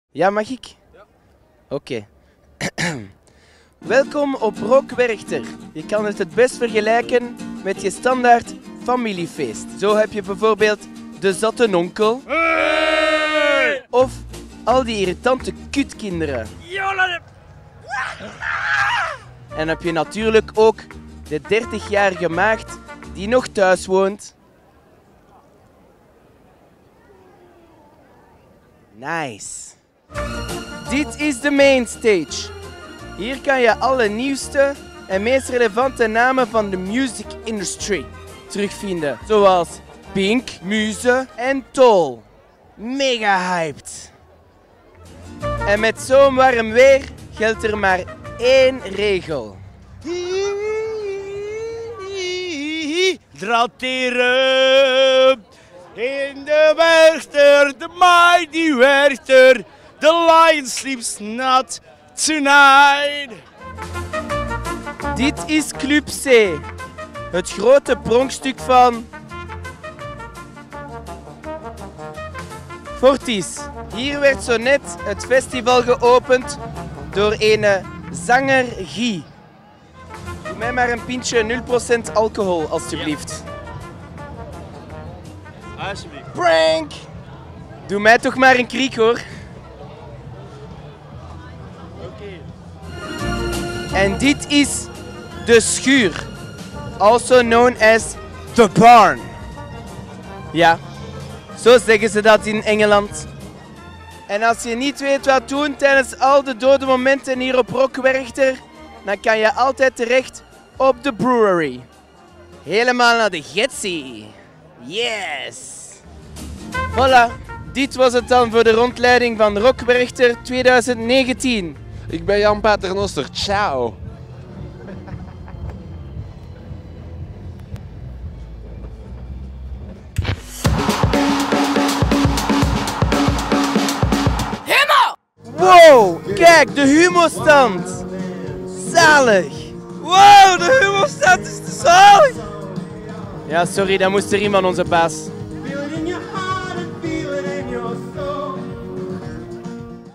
Average Rob leidt u rond op Rock Werchter 2019
'Je kan het festival het best vergelijken met een familiefeest: de zatte nonkel, de irritante kutkinderen en de 30-jarige maagd.'